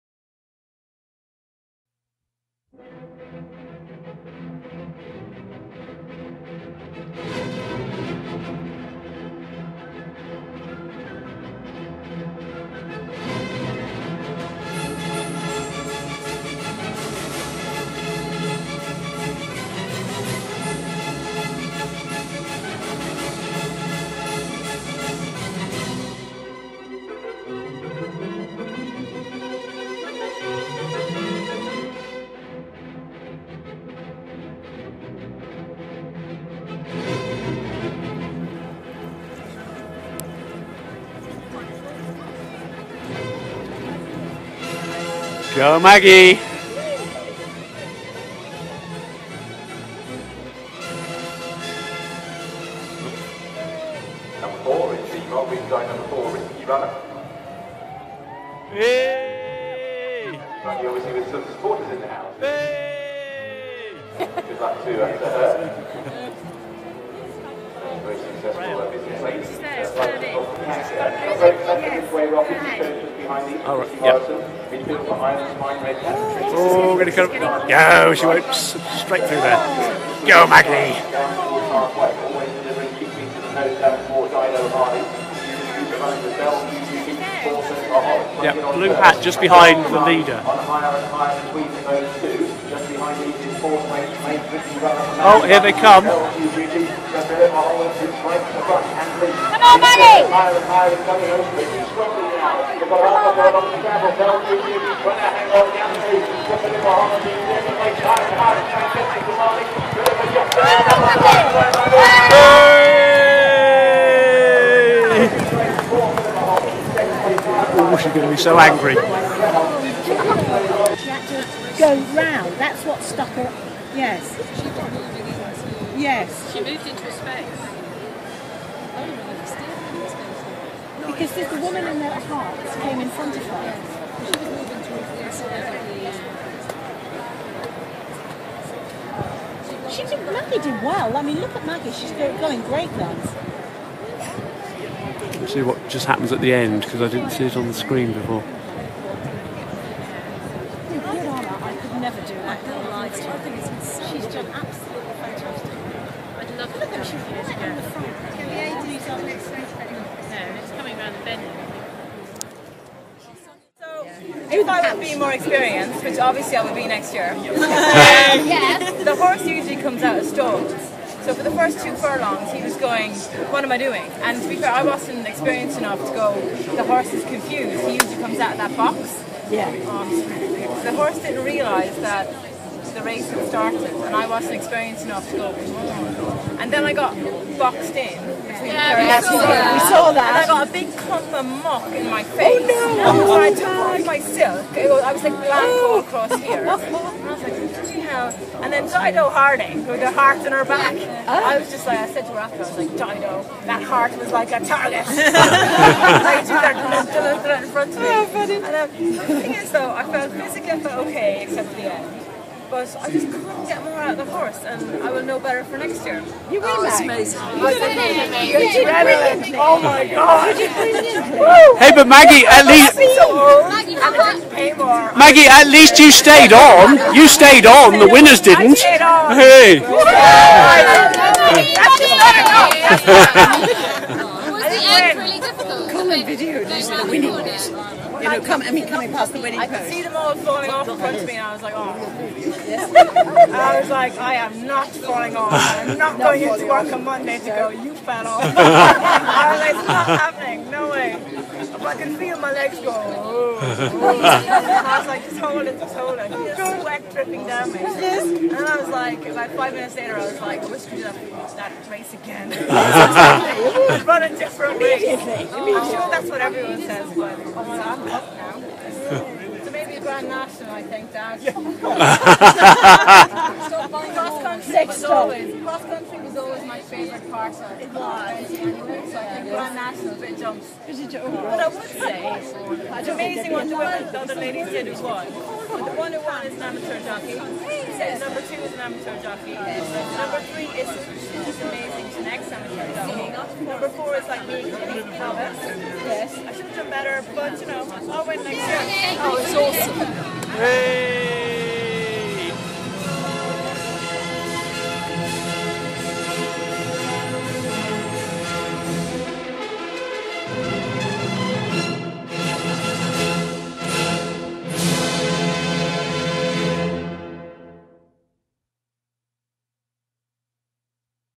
My horse racing debut for charity at the Magnolia Cup, Goodwood 2013
A fun video put together of the day